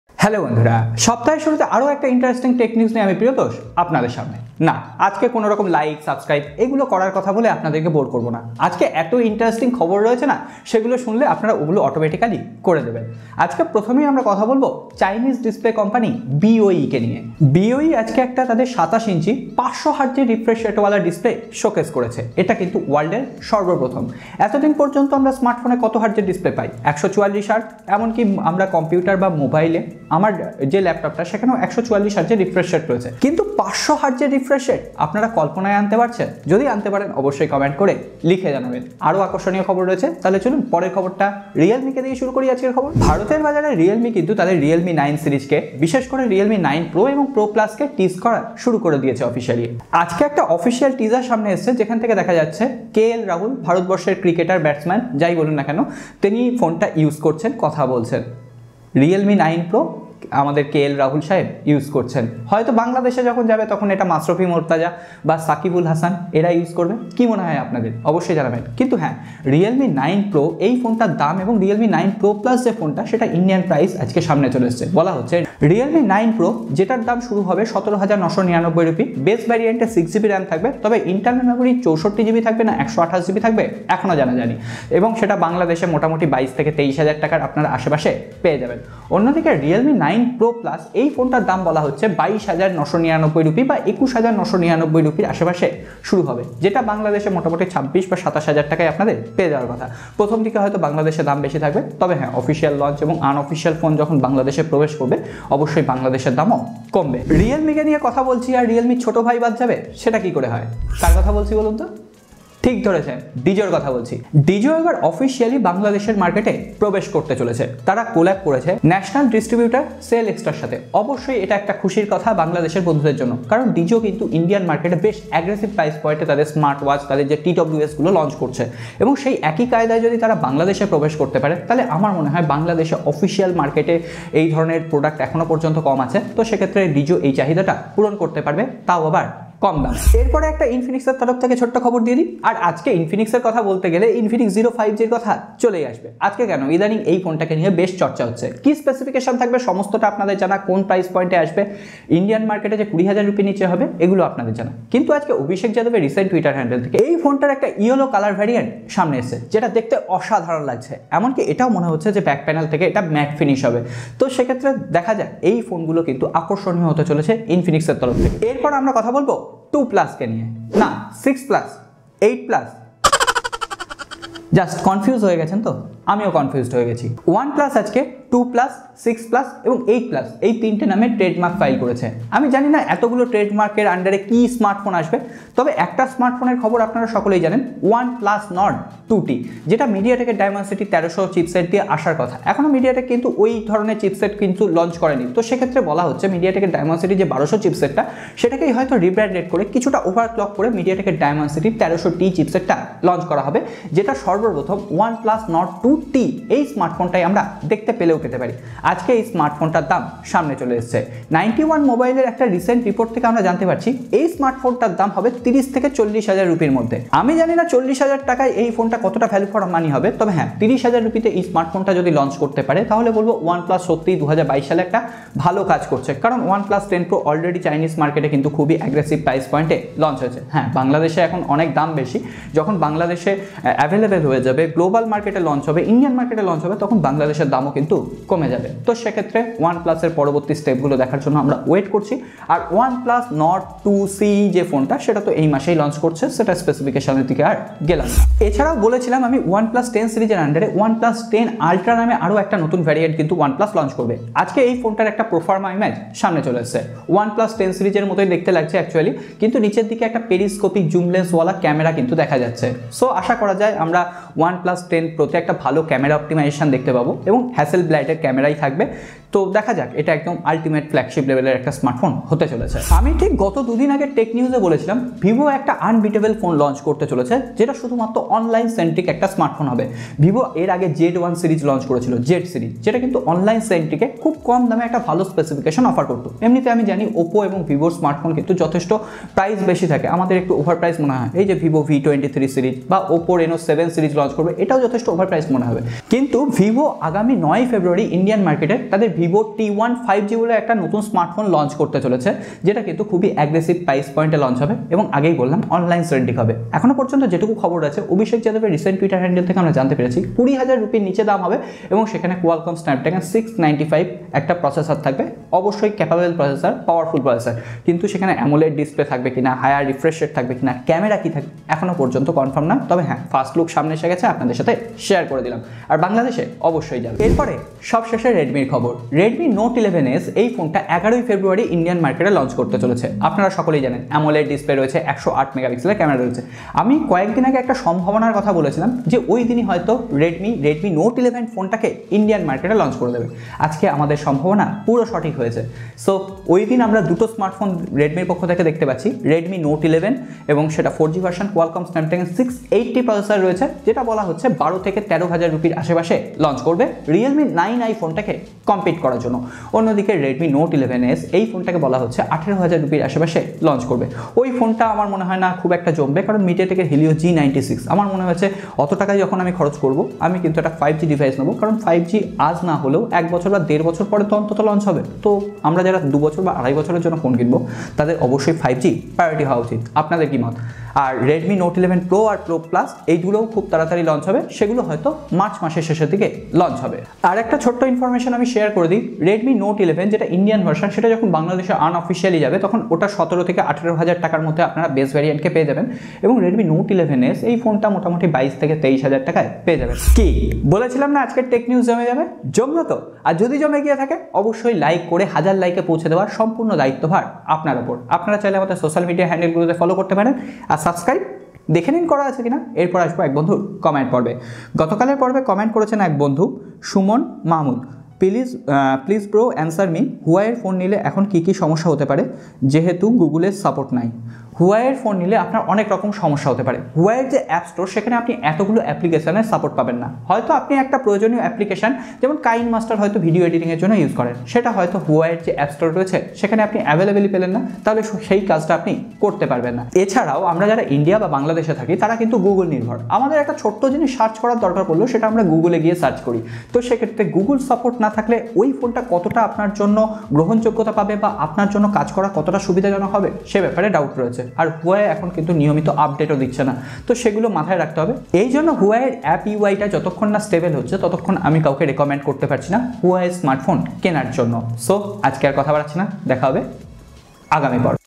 মধ্যবিত্তের আওতায় 1+ | Realme 9 Pro & Pro+ অস্থির দামে বাংলাদেশে 🔥 | Redmi Note 11 confirm price
মধ্যবিত্তের আওতায় 1+ | Realme 9 Pro & Pro+ বাংলাদেশ দাম 🔥 | Redmi Note 11 confirm Launch date,price | Tech News Episode : 458

Avail Amazon offer

BOE showcases the world’s highest refresh rate display, Realme 9 Pro & Realme 9 Pro+ Price, Dizo officially in Bangladesh, Infinix Zero 5G live image, OnePlus Nord 2T Price in India Leaked, OnePlus 10 Ultra design revealed, Oneplus Trademark, Vivo T1 5G India launch date, Vivo T1 5G - Snapdragon 695 - Under ₹20,000, Redmi Note 11, Redmi Note 11S Price in India Tipped,
Time Stamp :
Intro : 00:00
BOE 500 hz Display : 00:15
Realme : 00:45
Dizo Official : 01:50
Infinix Live : 02: 30
Oneplus : 02:40
Vivo T1 : 04:20
Redmi : 05:20
QNA : 06:30